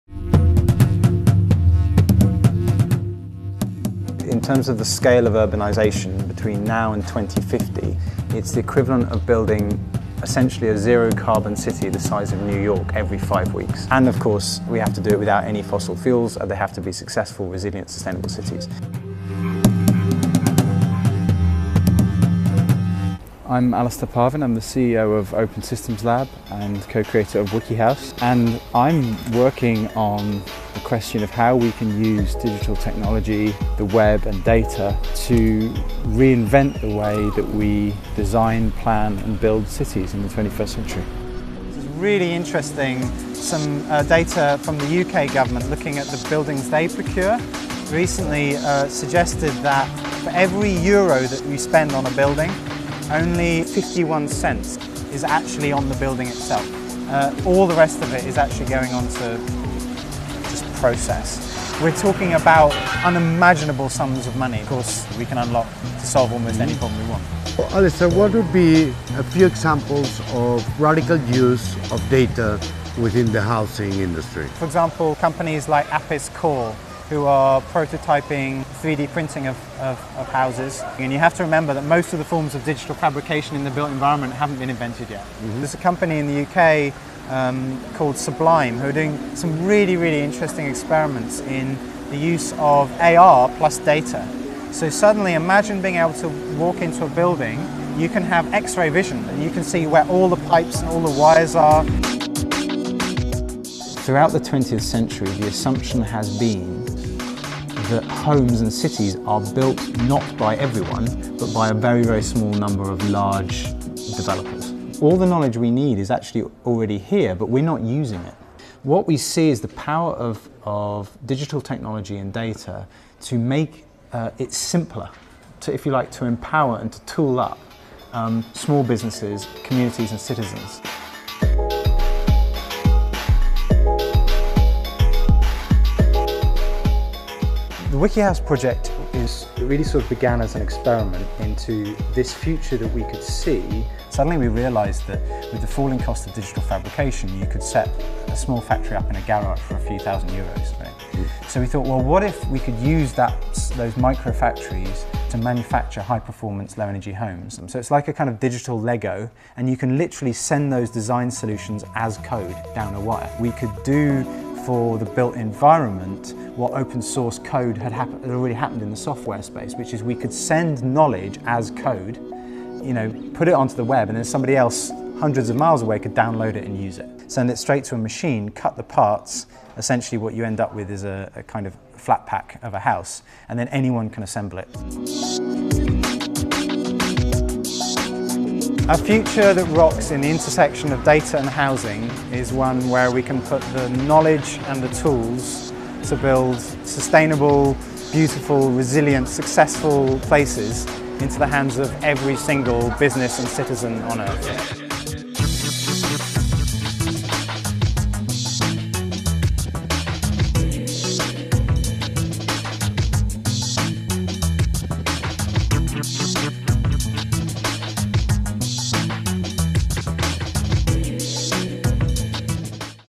Data Futures 12: Descarga los planos de tu casa y móntala como un mueble de Ikea
El código abierto ha irrumpido en la construcción de viviendas. Esta y otras tecnologías, como la impresión 3D o la realidad aumentada, permiten soñar en ciudades desarrolladas por los propios ciudadanos